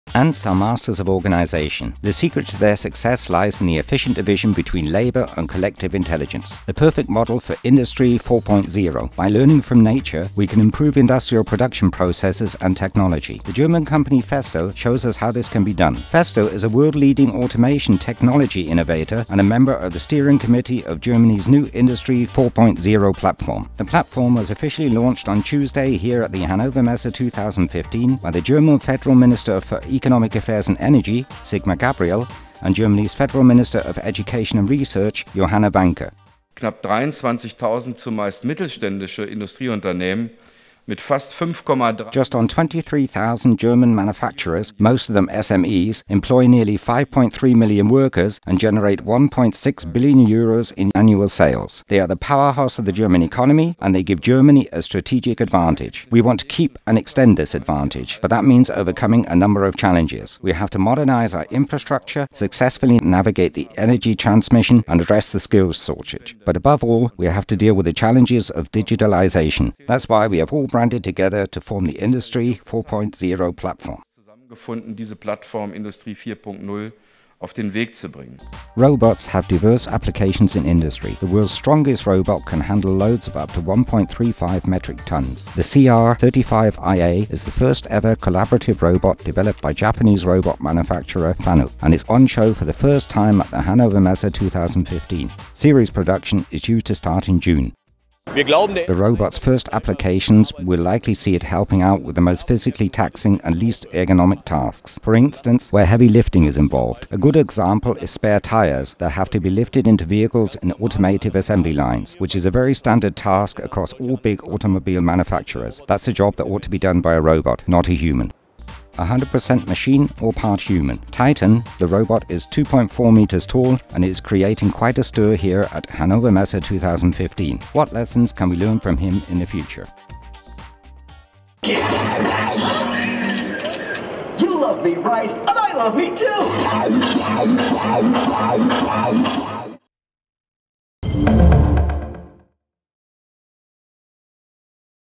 Industry 4.0 - The Industrial Revolution at HANNOVER MESSE 2015
HANNOVER MESSE: The perfect model for Industry 4.0. By learning from nature, we can improve industrial production processes and technology. The German company FESTO shows us at HANNOVER MESSE how this can be done.